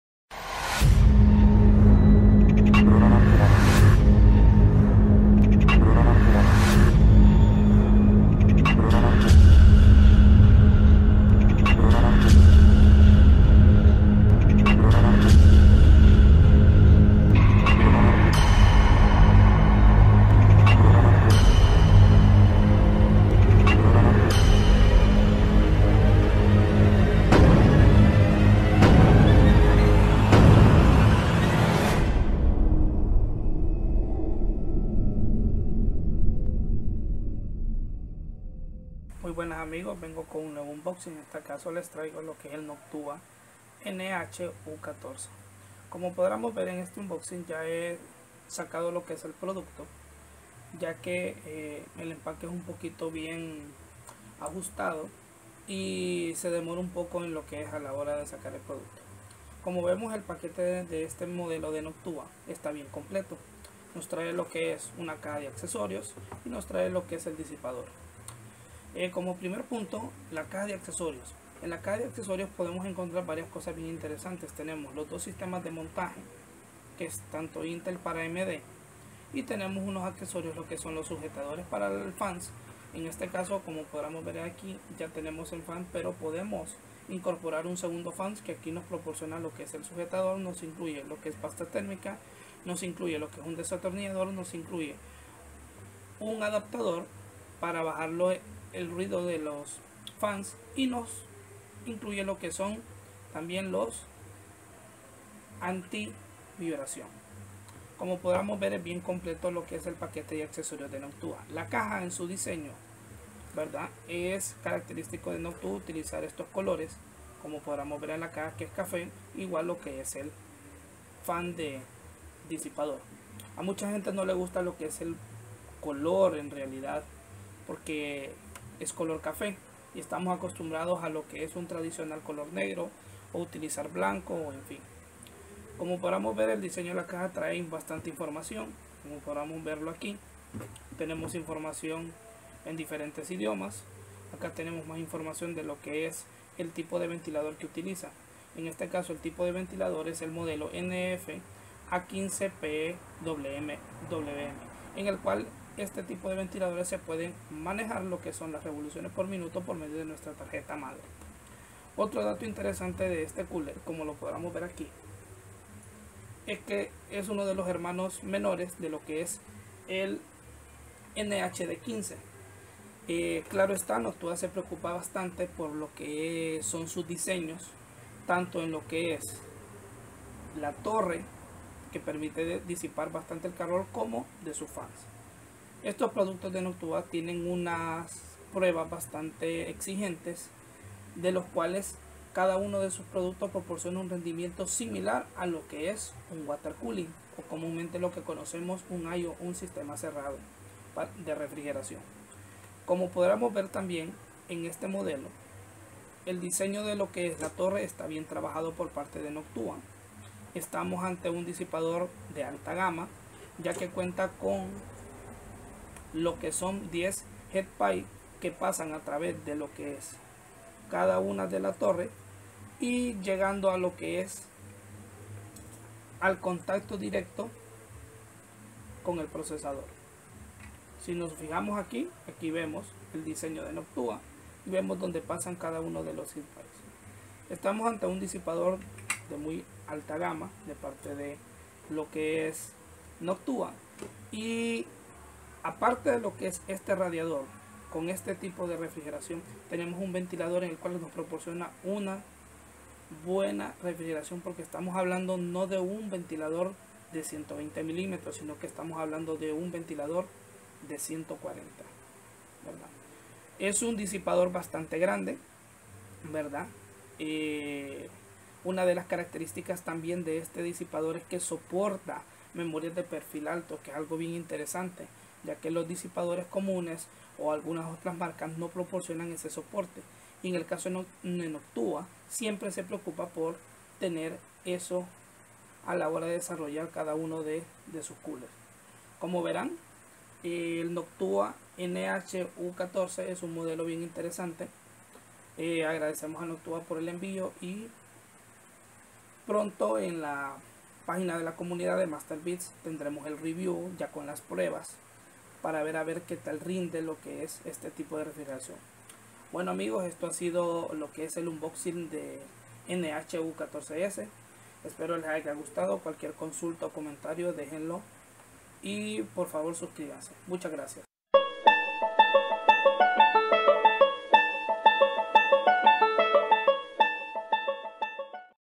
Unboxing Noctua NH U14S
Muy Buenas Amigos, nuevamente les traigo un unboxing y en esta ocasione presento a Noctua una marca bien interesante y con años en el mercado proporcionando Cooler con excelente Rendimiento, este modelo es bien interesante por su diseño y su fans de 140mm espero les guste y si no estan suscritos denle like y suscribanse.

Medios sociales